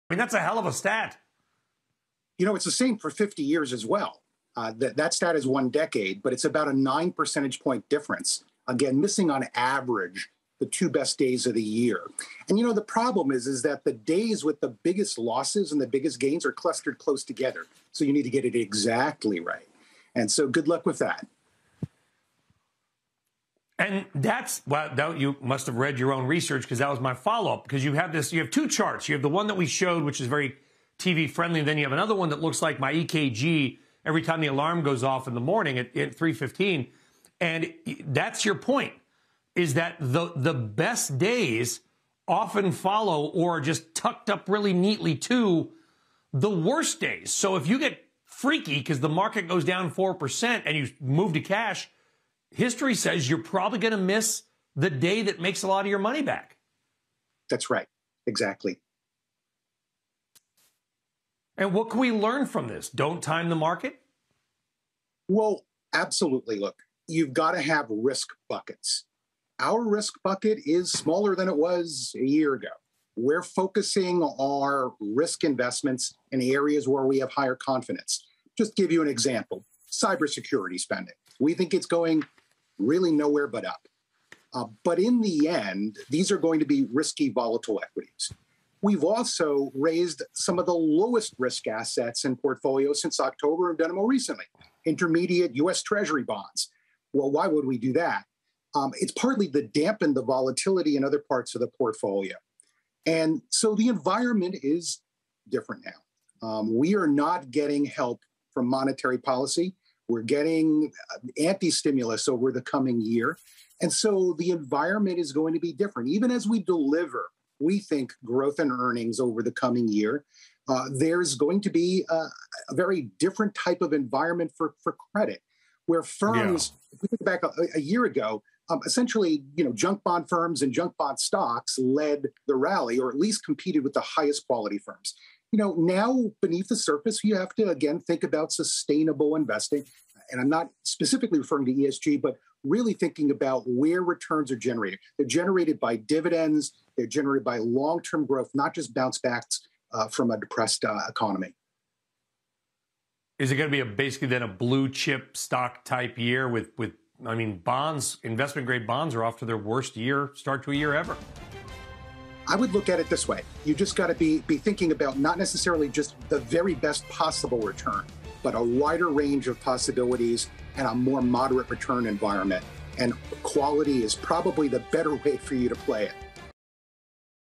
You can't time the market, you have to have 'risk buckets': Wieting
Steven Wieting of Citi Global Wealth Management discusses how investors should focus on areas of the market where there is higher confidence.